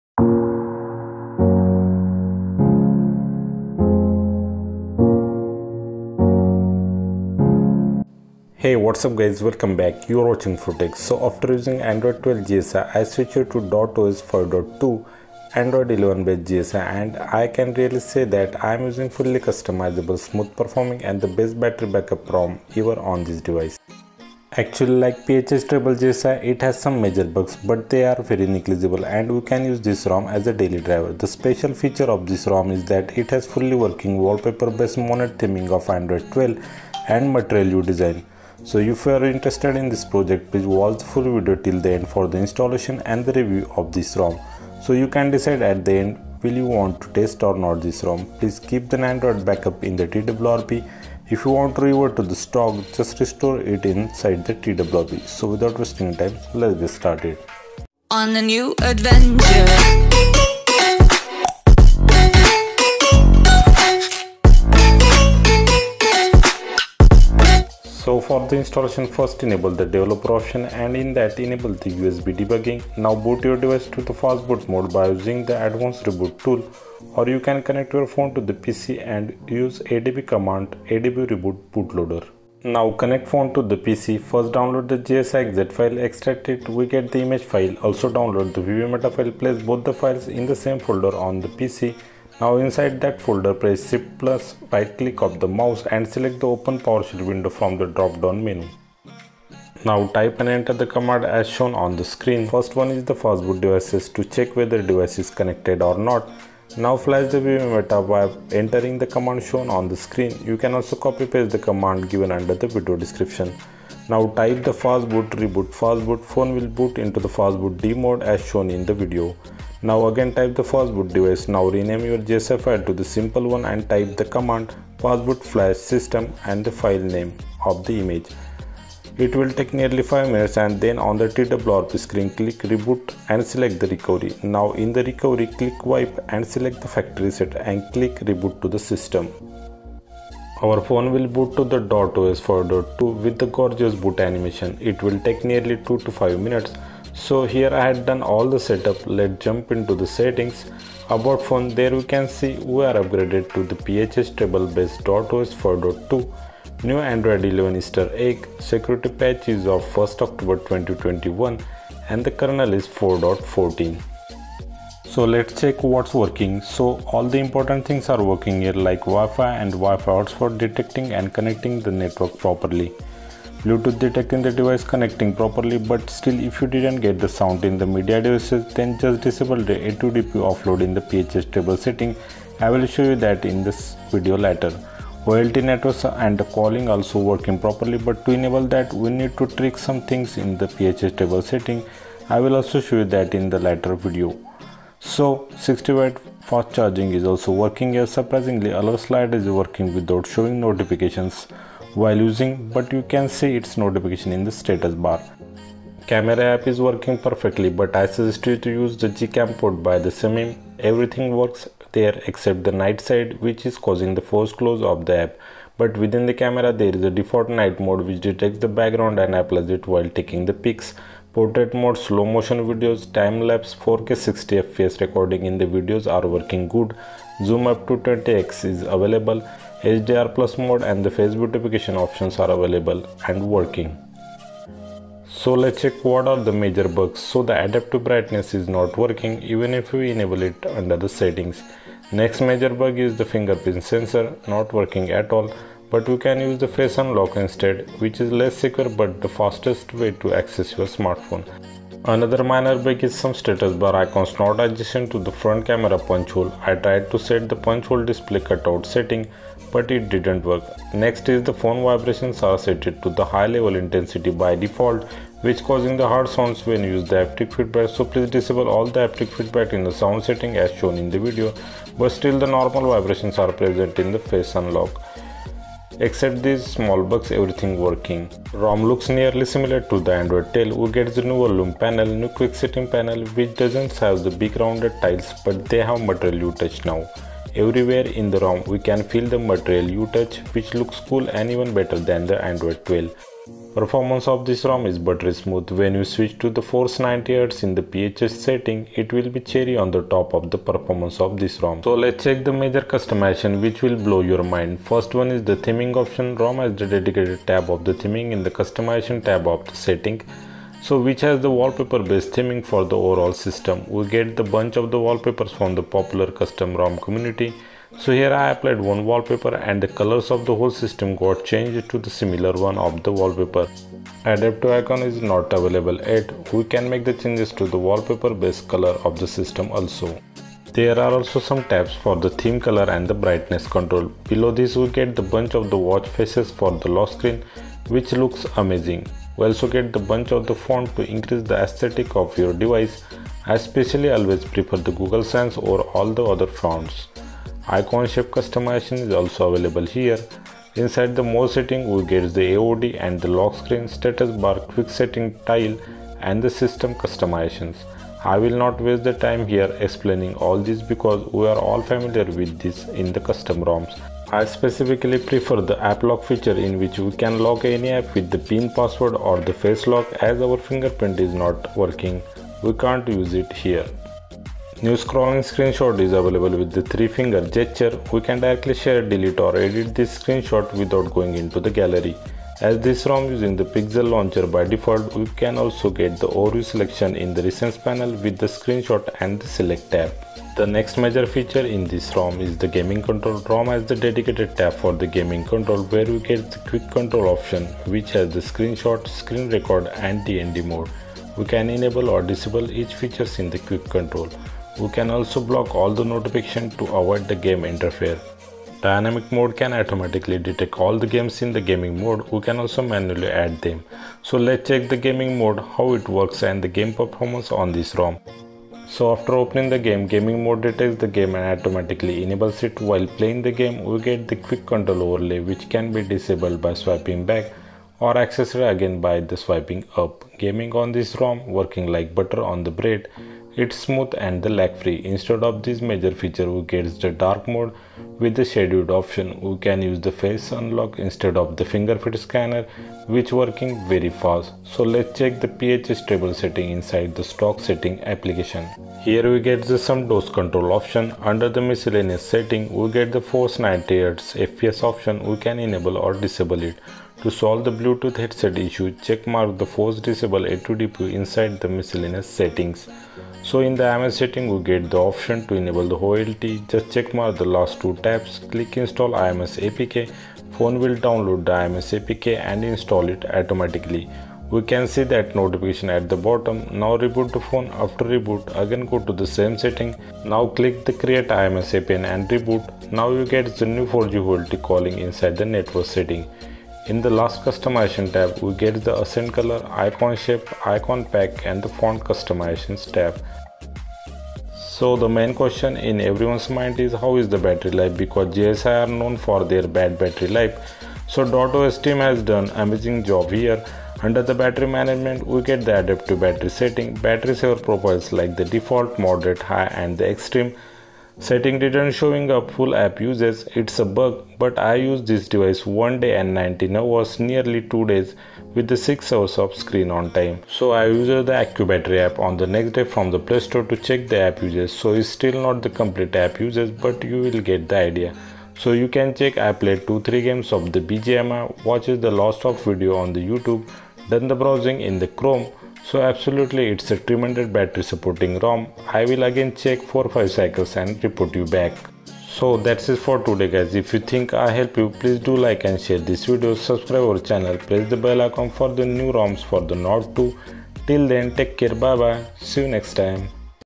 DotOS 5.2 Android 11 GSI OnePlus Nord 2: Better than PAC-MAN edition!
Download the rom(All files are available at our Blogger post)

Fastboot commands:

1) fastboot devices

2)to flash vbmeta:
fastboot --disable-verity --disable-verification flash vbmeta vbmeta.img

3)fastboot reboot fastboot

3)falsh DotOS gsi:

fastboot flash system gsifilename. Img